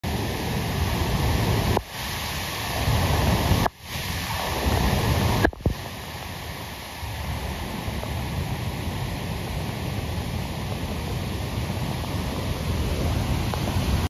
STRATH CREEK FALLS- 13 October 2022 Mt.Disappointment ,Victoria
Over the previous 7 days 185 mm of rain fell in this small catchment up in Mt. Disappointment State Forest. in the hour we were there a 11 mm of rain fell according to my rain gauge. videoed at 18:30 hours.